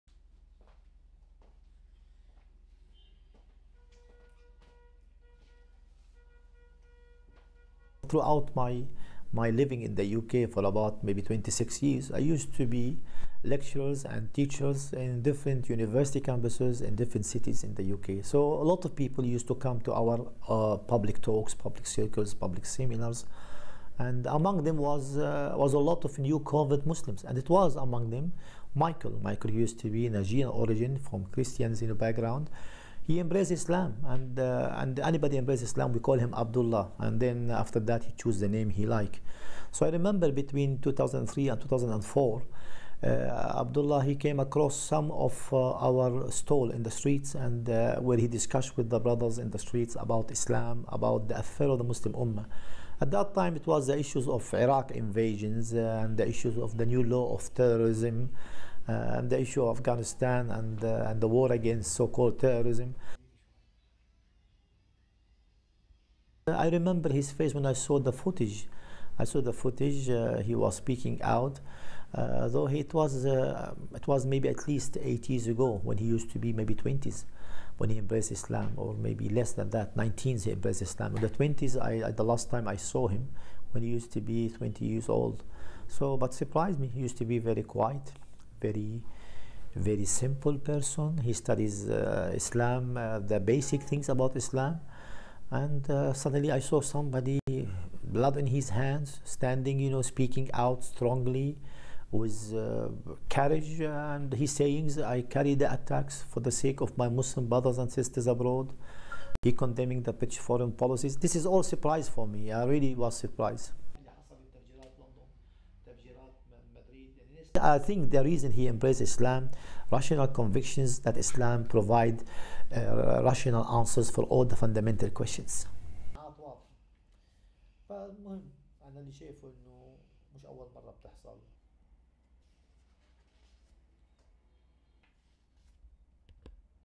Former radical preacher says he recognises suspect in attack of British soldier
1. Set-up of former radical Islamic leader Omar Bakri Muhammad at his Tripoli home
2. SOUNDBITE: (English) Omar Bakri Muhammad, Former radical Muslim leader:
"Throughout my living in the UK, for about maybe 26 years, I used to be lecturers and teachers in different university campuses and different cities in the UK. So a lot of people used to come to our public talks, public circles, public seminars, and among them were a lot of new convert Muslims, and it was among them Michael. Michael used to be Nigerian origin, from a Christian background. He embraced Islam and anybody who embraced Islam we call him Abdullah, and after that he chooses the name that he likes. So I remember that, between 2003 and 2004, Abdullah he came across some of our stalls in the street where he discussed with the brothers in the streets about Islam, about (inaudible) Muslim ummah (nation). At that time it was the issues of the Iraq invasion and the issue of the new law of terrorism, and they show Afghanistan and the war against so-called terrorism."
3. Cutaway of his hands
4. SOUNDBITE: (English) Omar Bakri Muhammad, Former radical Muslim leader:
"I remembered his face when I saw the footage. I saw the footage he was speaking out. Though it was maybe at least eight years ago when he was used to be maybe in his 20s when he embraced Islam, or maybe less than that, 19 when he embraced Islam, 20s the last time saw him, when he used to be about 20 years old. But it surprised me. He used to be a very quiet, very simple person. He studies Islam, the basic things about Islam, and suddenly I saw somebody ... blood on his hands, standing, speaking out strongly, with courage, and he is saying: 'I carried attacks for the sake of my Muslim brothers and sisters abroad', he is condemning the British foreign policy. This is all a surprise for me, I really was surprised."
8. Cutaway of flag
9. SOUNDBITE: (English) Omar Bakri Muhammad, Former radical Muslim leader:
"I think the reason he embraced Islam, rational convictions that Islam provides, rational answers for all the fundamental questions."
10. Various of Bakri at his home
STORYLINE:
A man suspected in the killing of a British soldier on the streets of London was described as a convert to Islam by a former radical Muslim leader on Thursday.
Omar Bakri Muhammad - who now lives in Lebanon but had been a radical Muslim preacher in London - said he recognised the man seen on television as Michael Adebolajo, a Christian who converted to Islam around 2003.
Police have not identified either of the two suspects and have not said when they would do so.
Authorities in Britain usually wait to name suspects after they have been charged.
Speaking from his Tripoli home on Thursday, Bakri described the suspect as someone who attended his London lectures in the early 2000s.
He said he "used to be Nigerian origin, from a Christian background" and that "he embraced Islam and anybody who embraced Islam we call him Abdullah."
"I remember that, between 2003 and 2004, Abdullah came across some of our stalls in the street where he discussed with the brothers in the streets about Islam," Bakri said.
"At that time it was the issues of the Iraq invasion and the issue of the new law of terrorism, and they show Afghanistan and the war against so-called terrorism."
Bakri said he remembers Adebolajo as a "quiet, simple person" who was keen to learn about Islam.
The two men suspected of killing the 25-year-old Lee Rigby, of the 2nd Battalion The Royal Regiment of Fusiliers, had been part of previous investigations by security services, a British official said on Thursday.